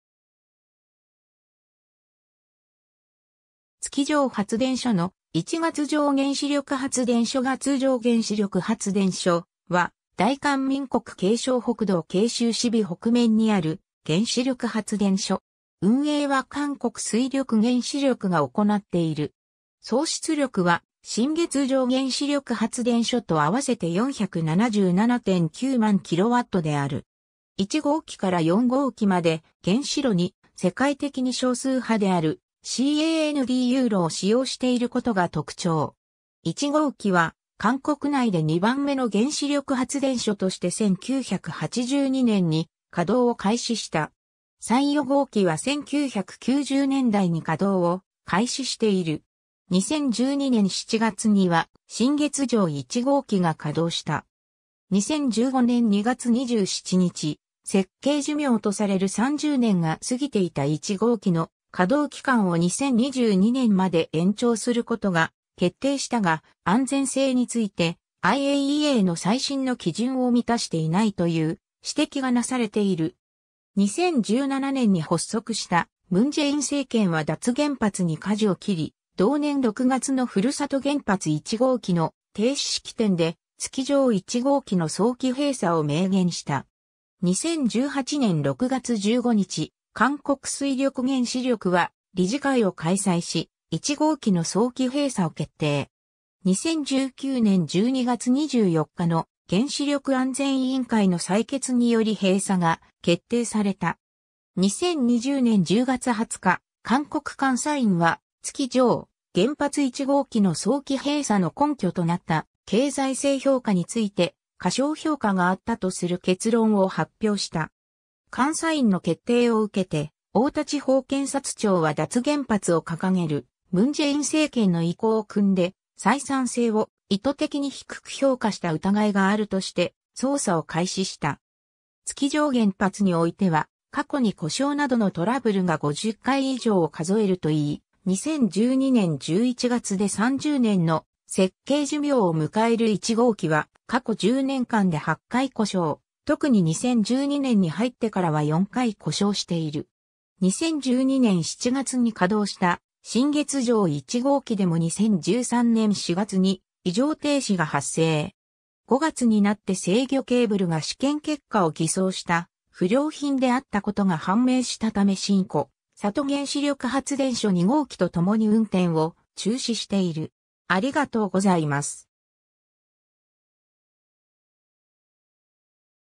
月城原子力発電所
月城 発電所の位置 月城原子力発電所 月城原子力発電所（ウォルソンげんしりょくはつでんしょ）は、大韓民国慶尚北道慶州市陽北面にある原子力発電所。
運営は韓国水力原子力がおこなっている。
総出力は新月城原子力発電所とあわせて477.9万kWである。
 1号機から4号機まで、原子炉に世界的に少数派であるCANDU炉（カナダ型重水炉）を使用していることが特徴。
1号機は、韓国内で2番目の原子力発電所として1982年に稼働を開始した。
3、4号機は1990年代に稼働を開始している。
2012年7月には新月城1号機が稼働した。
 2015年2月27日、設計寿命とされる30年が過ぎていた1号機の稼動期間を2022年まで延長することが決定したが、安全性についてIAEAの最新の基準を満たしていないという指摘がなされている。
2017年に発足した文在寅政権は脱原発に舵を切り、同年6月の古里原発1号機の停止式典で月城1号機の早期閉鎖を明言した。
2018年6月15日、韓国水力原子力は理事会を開催し、1号機の早期閉鎖を決定。
2019年12月24日の原子力安全委員会の採決により閉鎖が決定された。
 2020年10月20日、韓国監査院は月城原発1号機の早期閉鎖の根拠となった経済性評価について、過小評価があったとする結論を発表した。
監査院の決定を受けて、大田地方検察庁は脱原発を掲げる文在寅政権の意向を酌んで採算性を意図的に低く評価した疑いがあるとして、捜査を開始した。
 月城原発においては過去に故障などのトラブルが50回以上を数えるといい、2012年11月で30年の設計寿命をむかえる1号機は過去10年間で8回故障、特に2012年に入ってからは4回故障している。
 2012年7月に稼働した新月城1号機でも2013年4月に異常停止が発生。
5月になって制御ケーブルが試験結果を偽装した不良品であったことが判明したため新古里原子力発電所2号機とともに運転を中止している 。